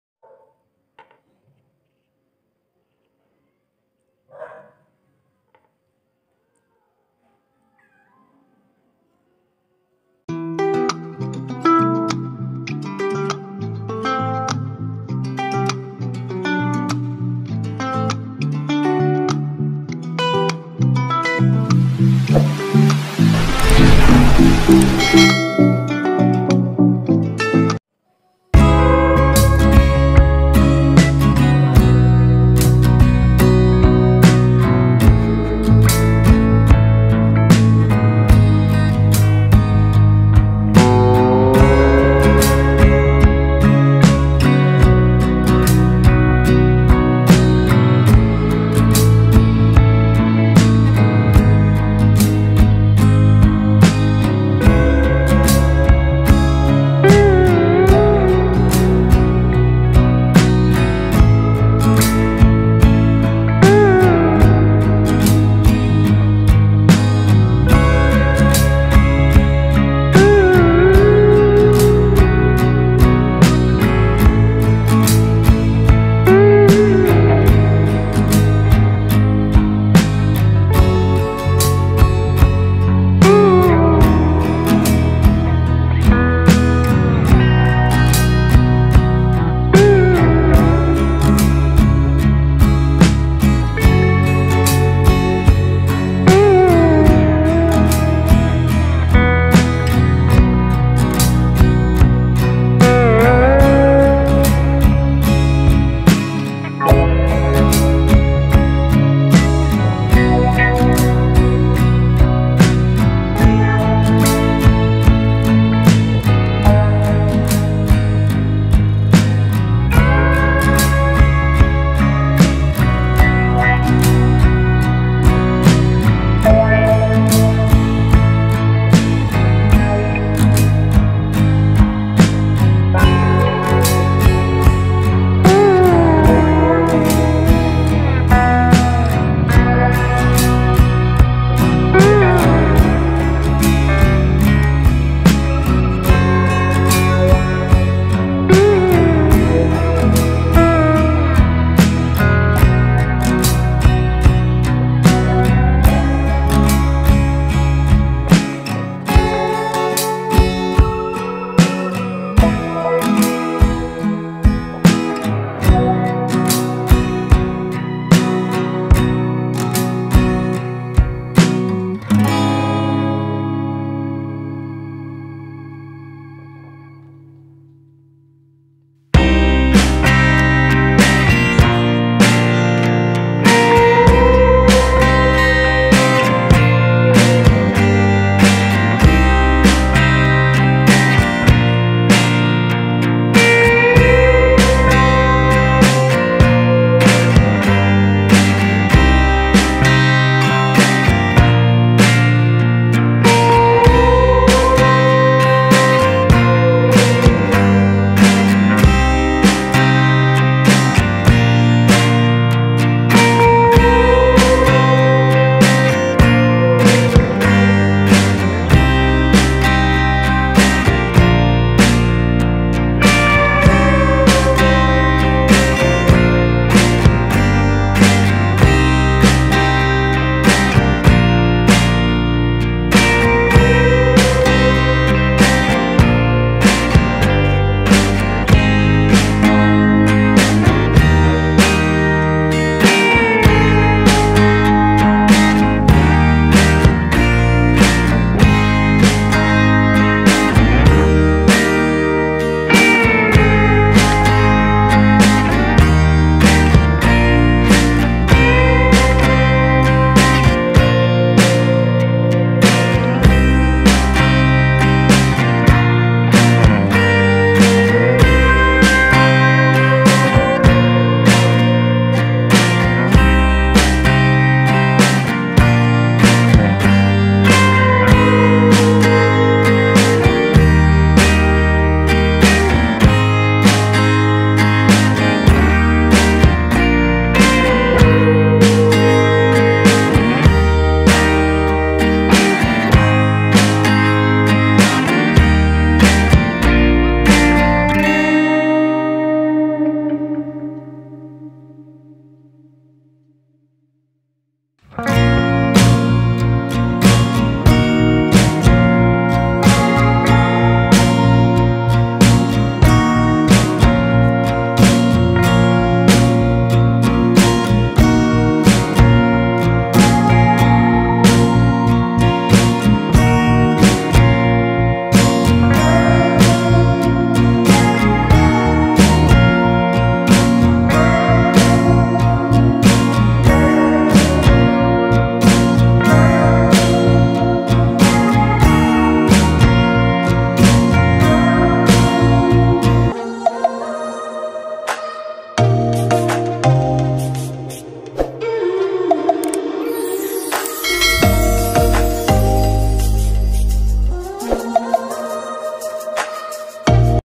Easy to cook Malunggay at Upo
healthy and budget tips Gulay para maging healthy tayo

INGREDIENTS:
upo
malunggay/Moronga
chicken
white onion
Garlic
Chicken cubes
salt
pepper
oil